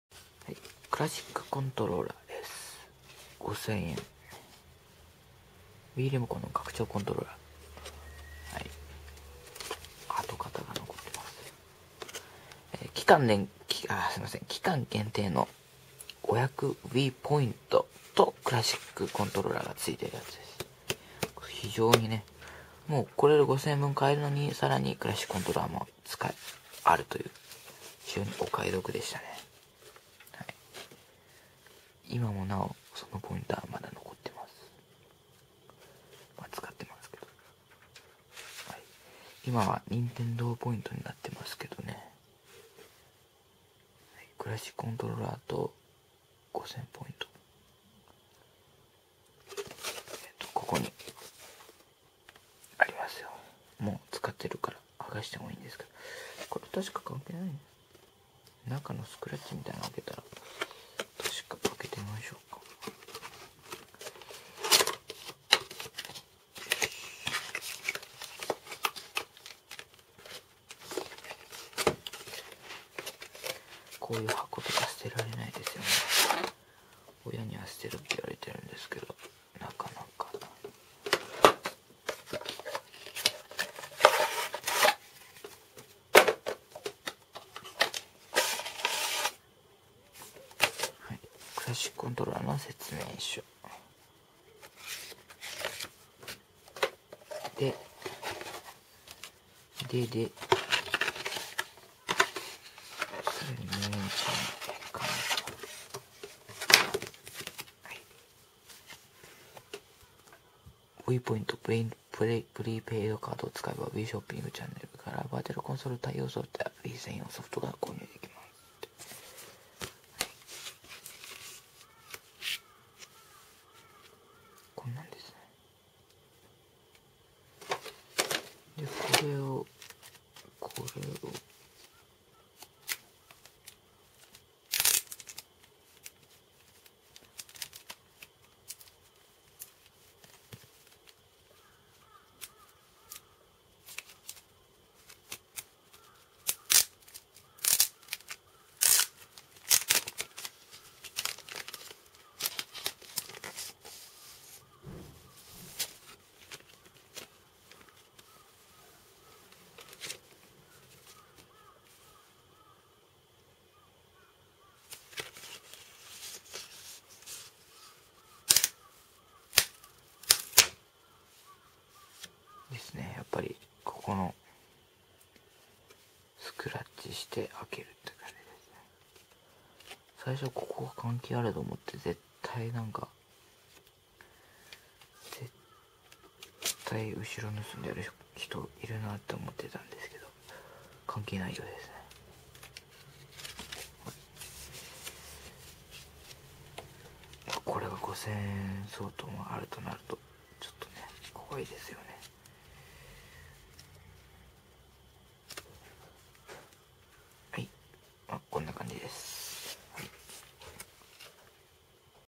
Nintendo Wii クラシックコントローラー with.5000WiiPoint 箱
NintendoのWii用「クラシックコントローラー with.5000WiiPoint」の箱ですw
クラコンもついてて5000Wiiポイントもついててかなりお得です！！